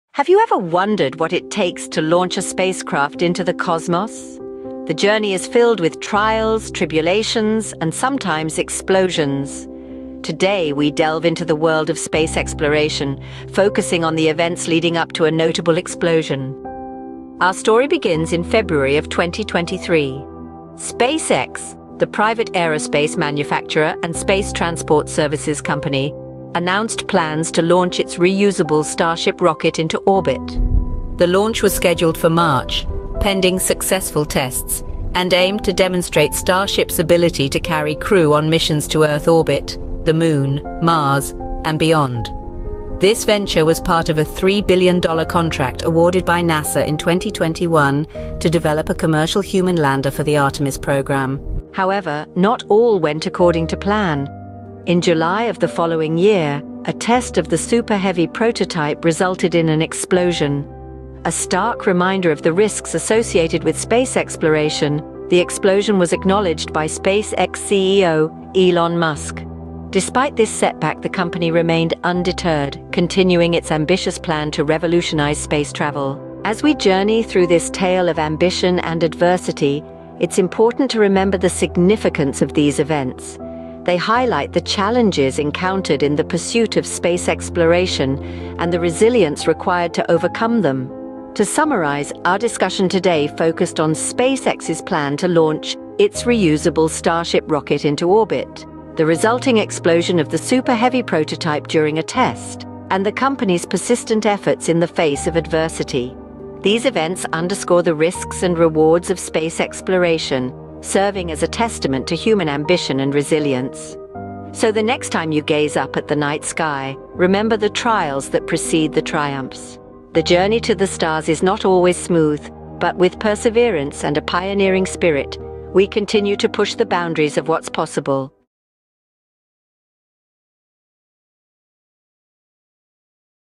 The SpaceX Starship Explosion: A Detailed Analysis
In this captivating video, we dive deep into the fascinating world of space exploration. Join us as we explore the SpaceX Starship explosion in 2020 and uncover its implications for the future of space travel. From the technical details of the explosion to the groundbreaking plans of SpaceX, we leave no stone unturned. With expert analysis and captivating visuals, this video promises to be an insightful journey for space enthusiasts and curious minds alike.

OUTLINE:

00:00:00 Launching the Starship Rocket
00:00:51 The Super Heavy Prototype Explosion
00:01:16 Ambition and Adversity